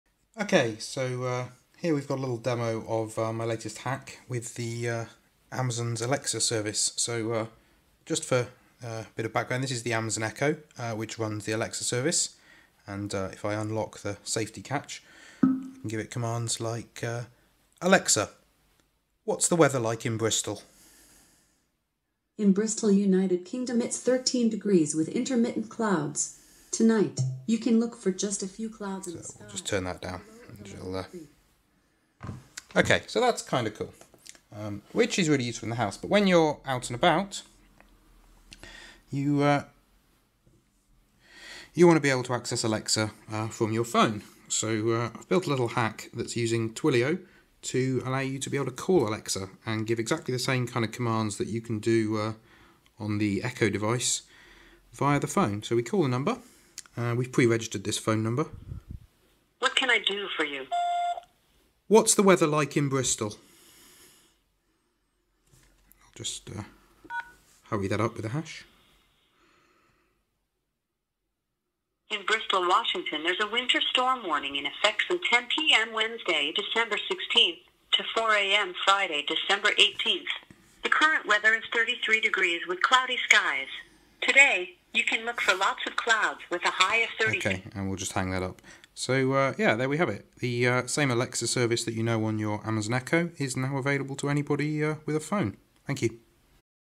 Call Alexa
Demo of my Alexa-Twilio hack, give alexa voice commands from your phone.